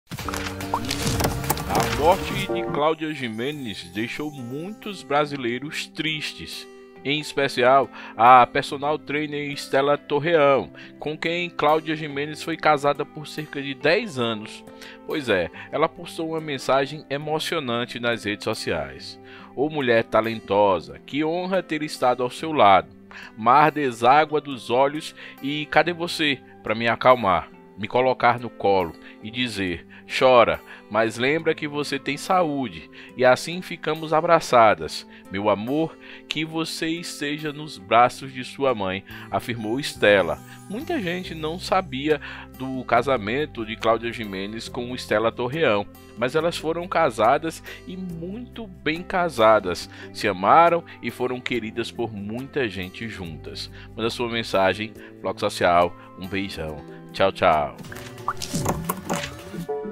ATITUDE DA ESPOSA DE CLAUDIA JIMENEZ NO VELÓRIO COMOVEU A DESPEDIDA DA ATRIZ DA GLOBO
A personal trainer Stella Torreão, com quem Claudia Jimenez foi casada por dez anos, postou no começo da tarde deste domingo (21) uma mensagem em homenagem à atriz. Claudia morreu no sábado (20).

“Ô mulher talentosa. Que honra ter estado ao seu lado; mar deságua dos meus olhos e cadê você para me acalmar, me colocar no colo, e dizer: ‘Chora...mas lembra que você tem saúde'. E assim ficávamos abraçadas. Meu amor, que você já esteja nos braços de sua mãe”, afirmou Stella na postagem.

A atriz estava internada no Hospital Samaritano, em Botafogo, na Zona Sul da cidade. Ela sofreu uma insuficiência cardíaca aos 63 anos. Claudia tinha sofrido um infarto em 1999 e, desde então, passou por três cirurgias no coração.